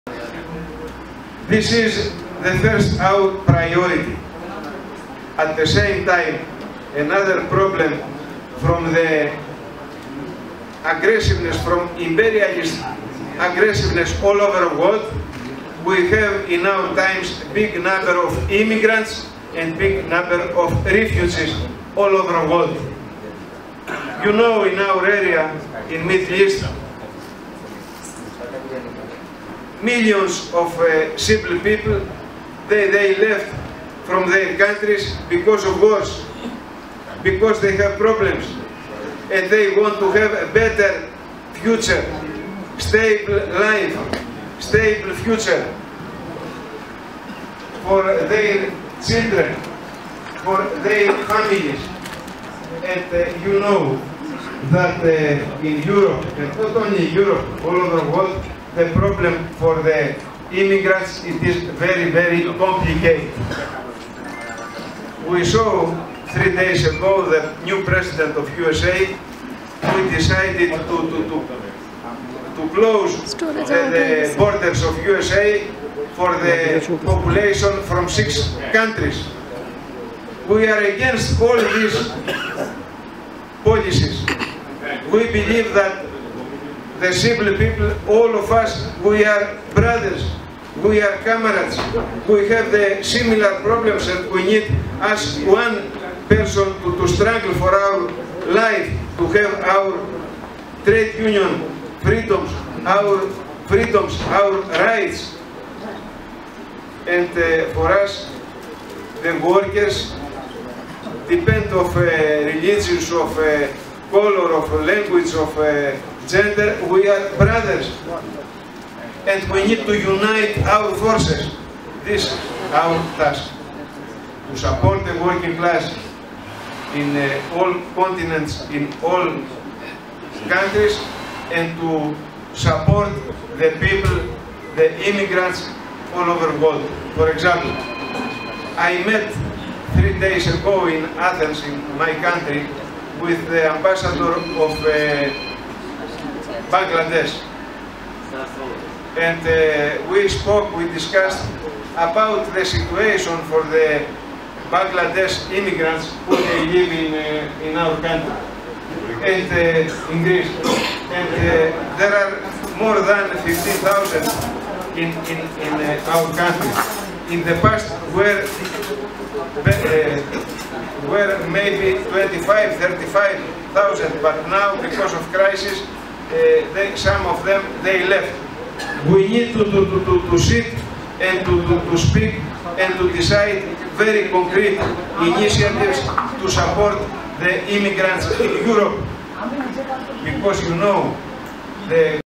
Visit of WFTU General Secretary in Bangladesh (meeting with affiliates and friends)
The WFTU General Secretary George Mavrikos is visiting Dhaka, Bangladesh from 3rd to 5th February 2017, invited by the WFTU affiliates and friends in the country. The WFTU General Secretary fulfilled his promise to the workers of Bangladesh during the 17th World Trade Union Congress to visit the country and discuss analytically with all the WFTU affiliates about the common initiatives that can be taken in national and sectoral level.